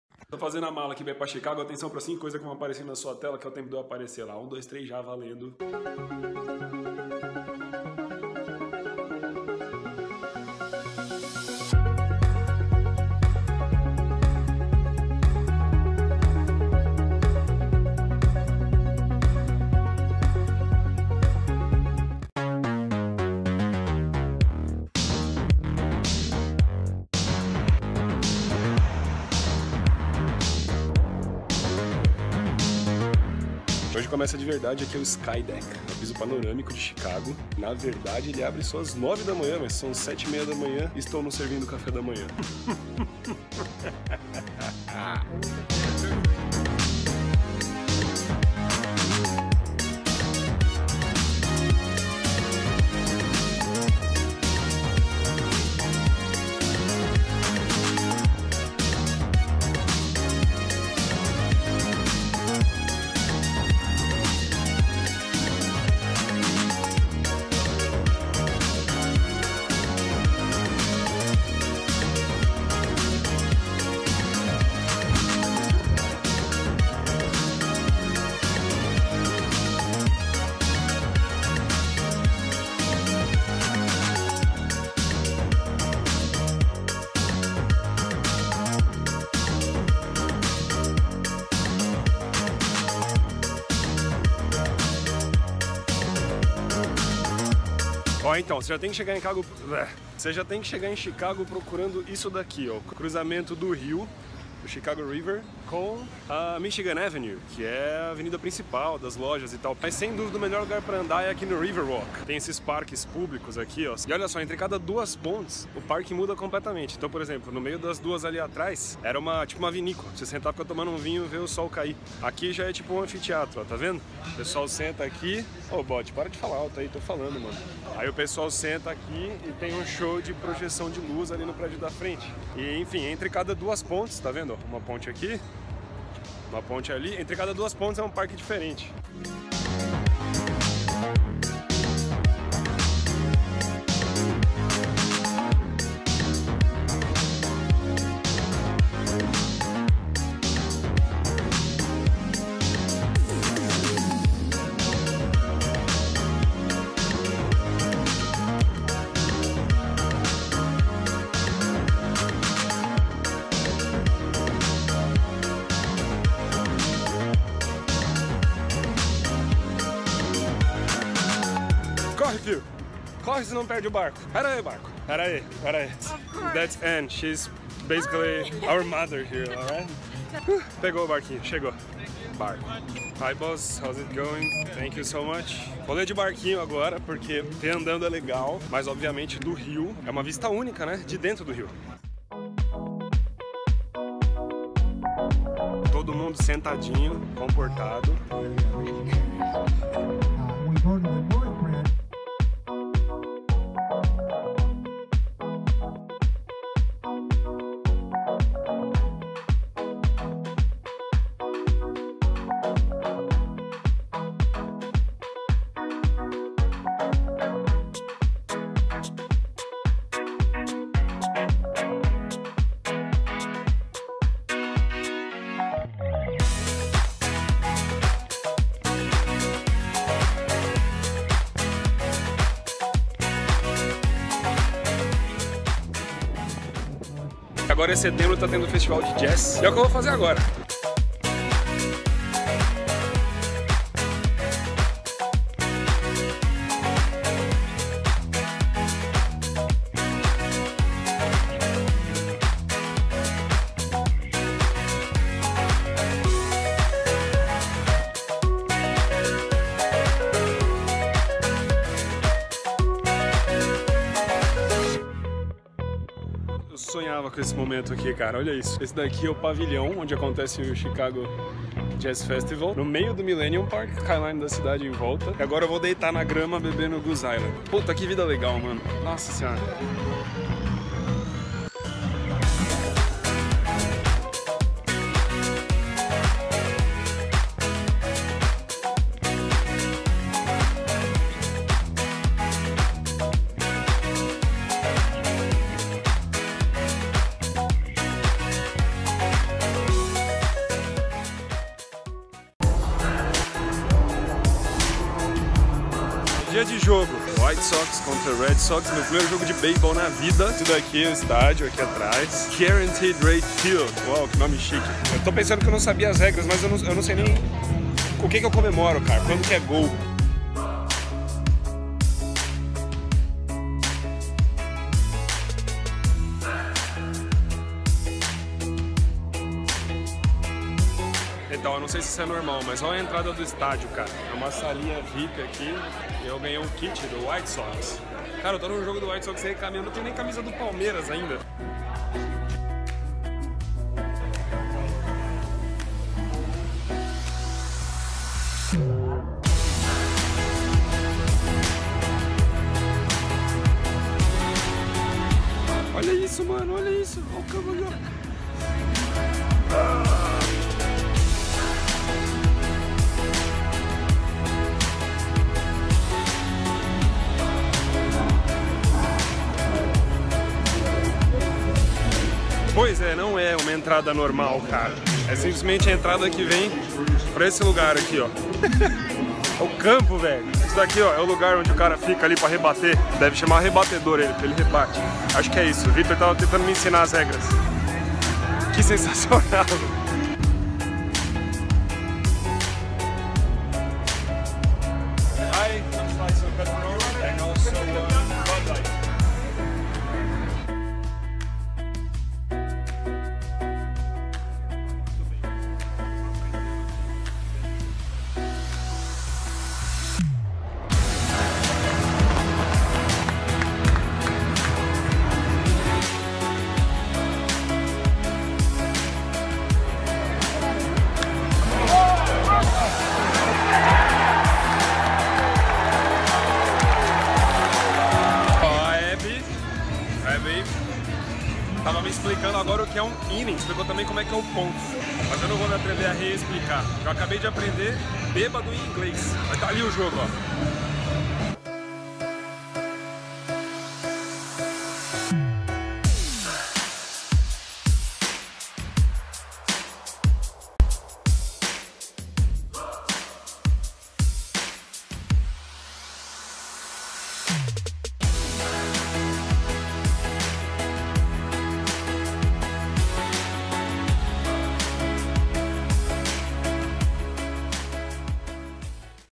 Chicago - EUA :: O que fazer para ter um dia inesquecível em Chicago :: 3em3
Neste episódio, visitamos:
1) Skydeck
2) Chicago Riverwalk
3) First Lady Cruises
4) Millenium Park
5) Jogo de Baseball

Imagens: Tiago Caramuru
Texto: Tiago Caramuru
Edição: Tiago Caramuru